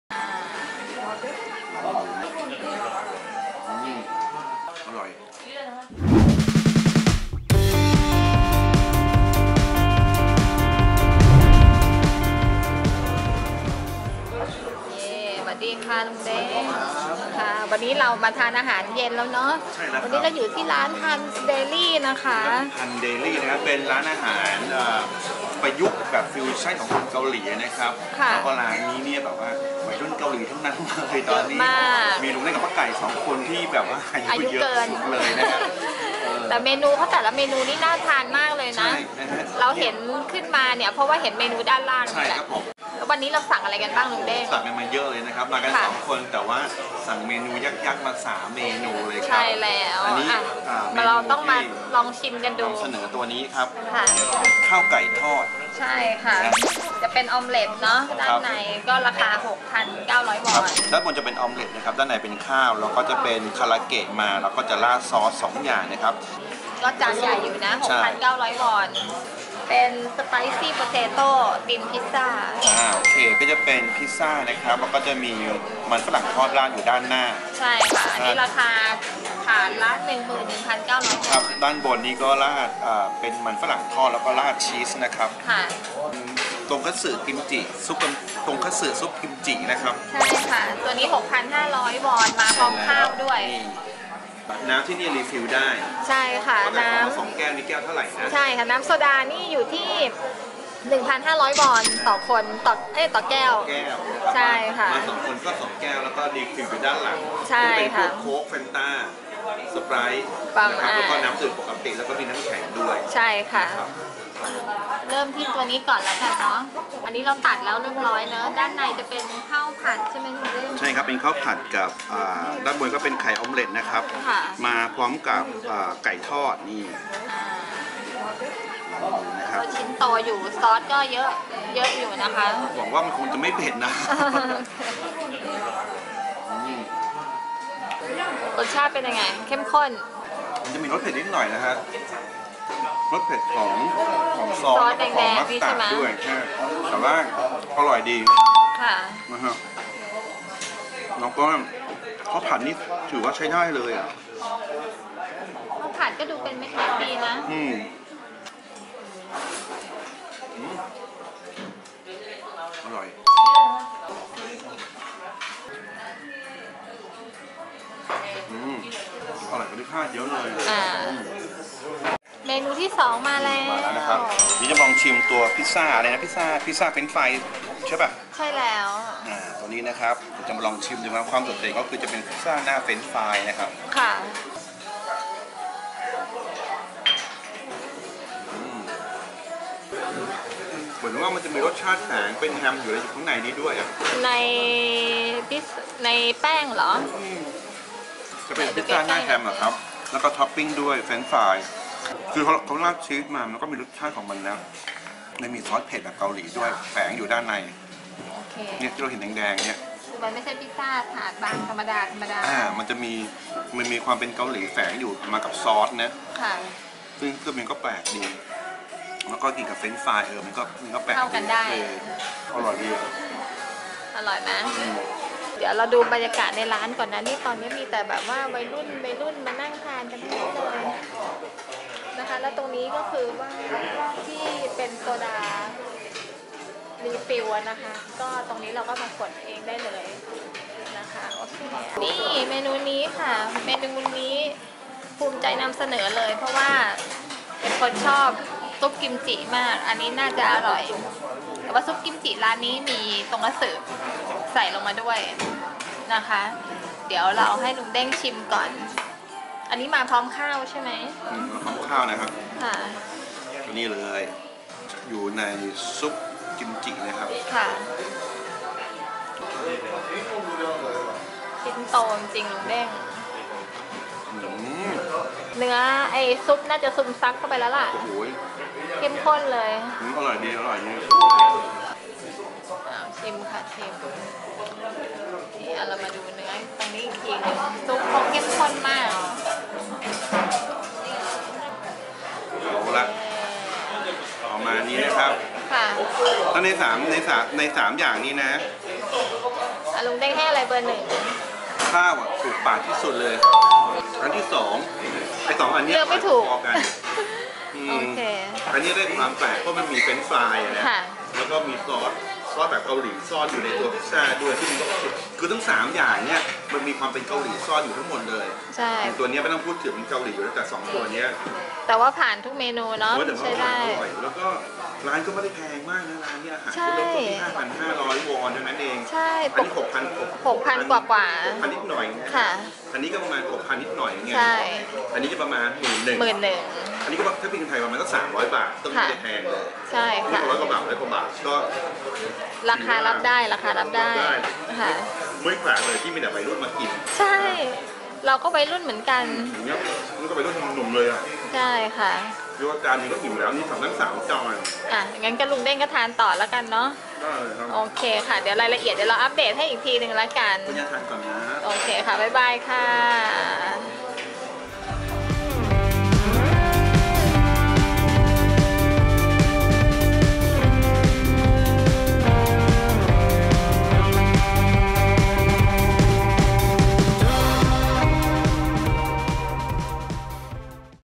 เที่ยวเกาหลี : แทกู (Daegu EP.7) Han’s Deli ร้านอาหารถูกใจวัยรุ่นเกาหลี
Han’s Deli อาหารสนุกๆ ถูกใจวัยรุ่นเกาหลี อร่อย ราคาประหยัด ย่าน ดงซองโน เมือง แดกู

รีวิวเมืองแทกู
รวมร้านอาหารอร่อยเมืองแทกู
รวมสถานที่ท่องเที่ยวเมืองแทกู

Korea Fan Club
ติดตามข้อมูลท่องเที่ยวเกาหลีได้ตามลิงค์ข้างล่างนี้